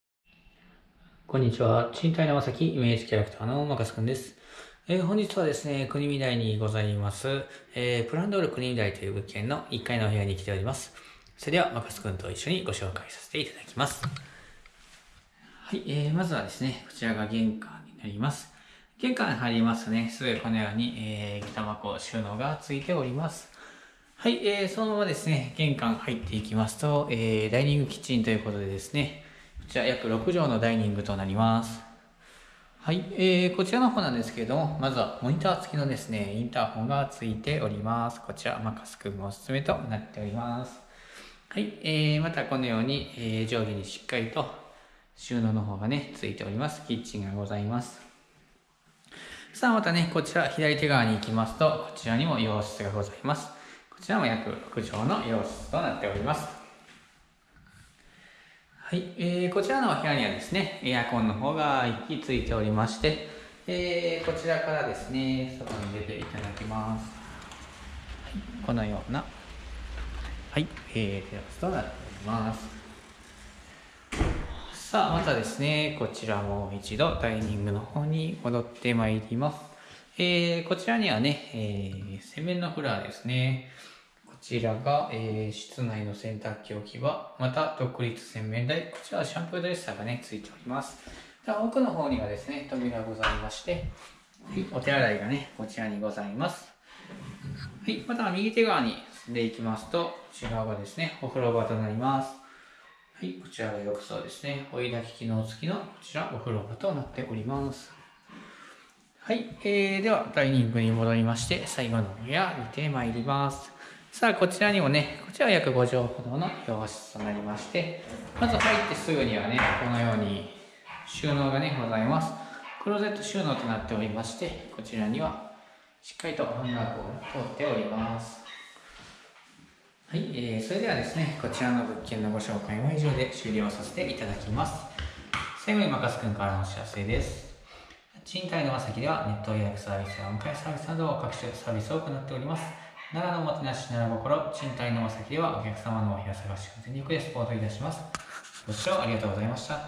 3369ブランドール州見台101内観 案内動画♪賃貸のマサキ
京都府木津川市にあります２DKアパートの【ブランドール州見台】の紹介動画になります。